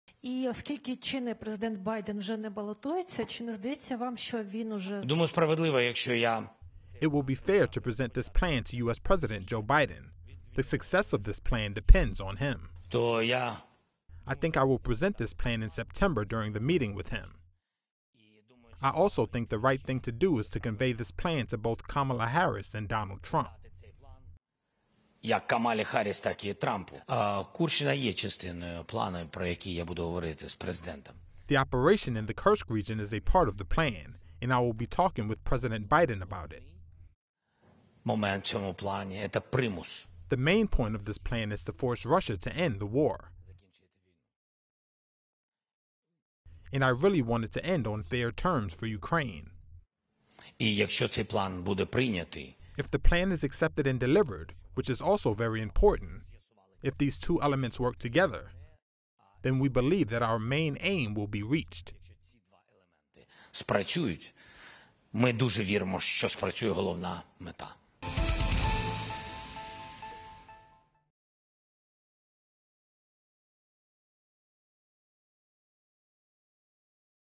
Zelenskiy to Show Biden Plan to End War with Russia | News9
Ukraine's Volodymyr Zelenskiy said on Tuesday (August 27) that the war with Russia would eventually end in dialog, but that Kyiv had to be in a strong position and that he would present a plan to U.S. President Joe Biden and his two potential successors.